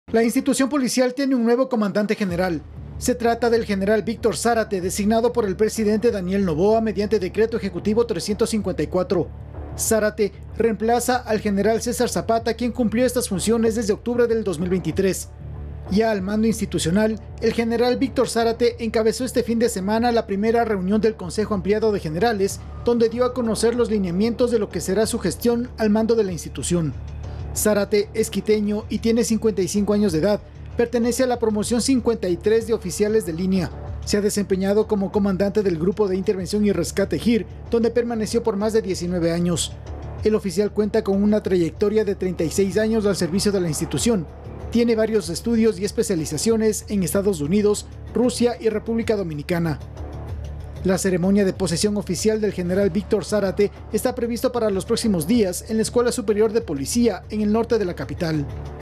Noticias7 - VÍCTOR ZÁRATE COMO NUEVO COMANDANTE GENERAL DE LA POLICÍA NACIONAL
Noticias7 - DIRIGIR EL PRIMER CONSEJO AMPLIADO DE GENERALES FUE LA PRIMERA ACTIVIDAD QUE CUMPLIÓ EL GENERAL VÍCTOR ZÁRATE COMO NUEVO COMANDANTE GENERAL DE LA POLICÍA NACIONAL...

ZÁRATE ES UN OFICIAL CON 36 AÑOS DE SERVICIO EN LA INSTITUCIÓN POLICIAL Y CUENTA CON VARIOS CURSOS DE ESPECIALIZACIÓN EN ESTADOS UNIDOS, RUSIA Y REPÚBLICA DOMINICANA...